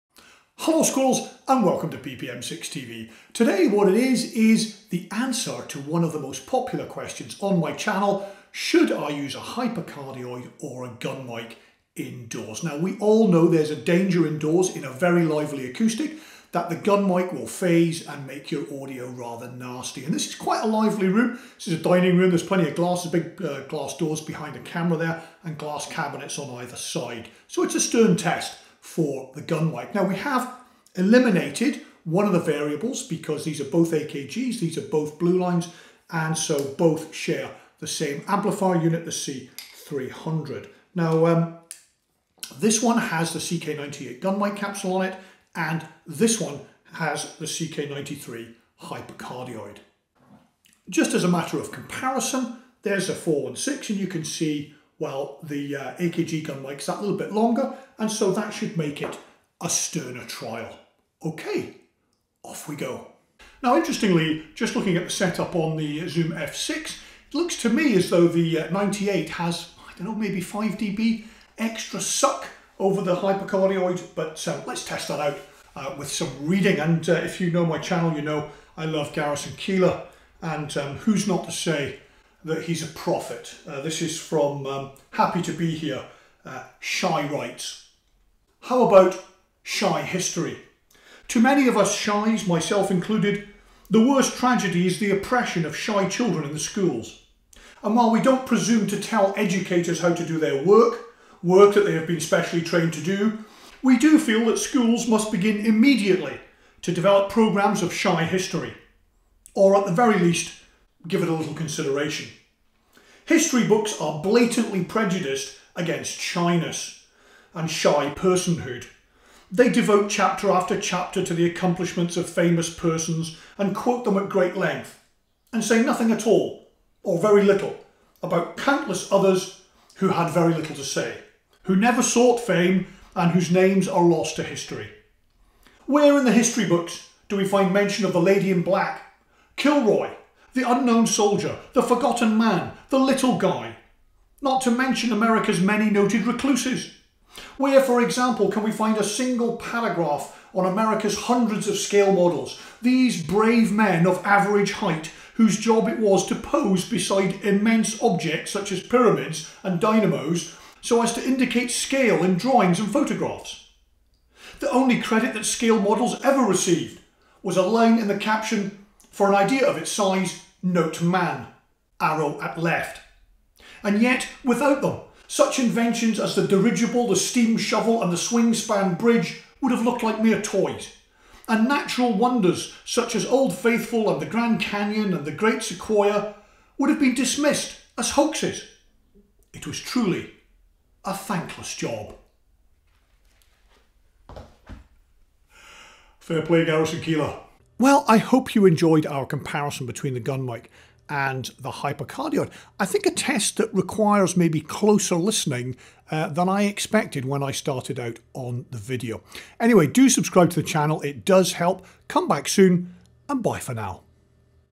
Are gun mics rubbish indoors? Myth or Magic. Gun V Hypercardioid AKG CK98 v CK93.
Many sound recordists have angst about using a gun mic indoors. But is it really a problem? Do you have to buy an expensive (Schoeps or Neumann!)  hypercardioid or super cardioid and leave your Sennheiser 416 at the door? We try AKG's CK98 rifle mic against it's little brother the hypercardioid CK93

00:38 SE 300B
00:50 CK98 v Sennheiser MKH 416
01:05  Test set up
01:11 CK93
01:15 CK98
01:20 Shy Rights